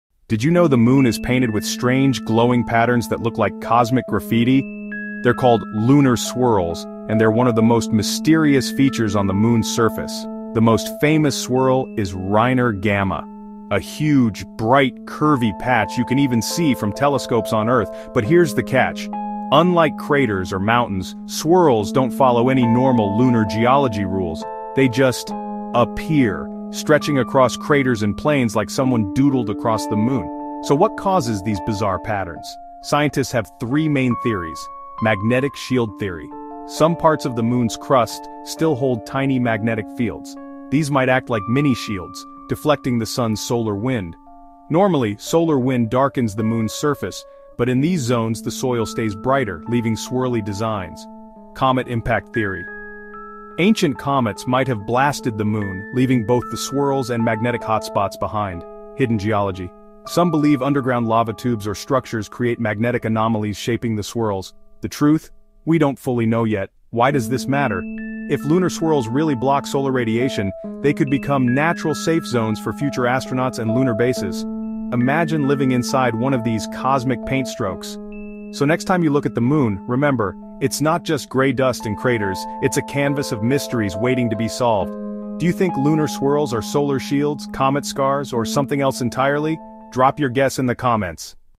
🌙 Lunar Swirls – The Mysterious Patterns on the Moon’s Surface | Explained in Detail 🌙
Have you ever looked at the Moon and wondered about its strange, glowing patterns? These fascinating, ghostly marks are known as **lunar swirls**—some of the most mysterious and breathtaking features on the Moon. Unlike craters or mountains, lunar swirls don’t seem to belong to any obvious geological process, leaving scientists and space enthusiasts puzzled for centuries.

In this video, we dive deep into the mystery of **lunar swirls**, exploring their appearance, history, possible origins, and why they are so important for the future of space exploration. From the famous **Reiner Gamma swirl** to other bright, curving signatures scattered across the lunar surface, these patterns are unlike anything else in our solar system.

✅ What are **lunar swirls** and how do they look?
✅ The story of their **discovery** through telescopes and spacecraft.
✅ Top scientific theories – from **magnetic shielding** against solar wind to **comet impacts** and underground geology.
✅ Why NASA and scientists are deeply interested in studying swirls.
✅ How **lunar magnetic anomalies** may protect future astronauts from solar radiation.
✅ What these swirls can teach us about the **Moon’s history** and planetary evolution.

✨ **Why Are Lunar Swirls Important?**
Lunar swirls are not just pretty patterns—they could be natural laboratories for understanding how cosmic forces interact with planetary surfaces. If magnetic fields at these sites really shield the surface from radiation, then swirls may hold the key to building future **lunar bases** and supporting **long-term human missions** on the Moon. Imagine astronauts living under the natural protection of a lunar swirl!

🌌 **Theories Behind the Mystery:**
1️⃣ **Magnetic Shield Theory** – Small, localized magnetic fields deflect solar wind, leaving bright swirls.
2️⃣ **Comet Impact Theory** – Charged particles from ancient comet impacts created magnetic hotspots and swirling designs.
3️⃣ **Geological Mystery** – Possible links to hidden underground features like lava tubes.

Even today, no single theory fully explains them, making lunar swirls one of the greatest unsolved mysteries of lunar science.

If you’re fascinated by the Moon, space exploration, or cosmic mysteries, this video is for you. We explain everything in a clear, engaging way—perfect for students, space enthusiasts, and anyone who loves uncovering the secrets of the universe.

By the end of this video, you’ll see the Moon not just as a gray, cratered world, but as a canvas painted with cosmic patterns that scientists are racing to understand.

💡 **Join the Cosmic Conversation!**
👉 Do you think lunar swirls are nature’s artwork, or the result of some unknown cosmic event?

🔎 **Keywords for Ranking (naturally included):**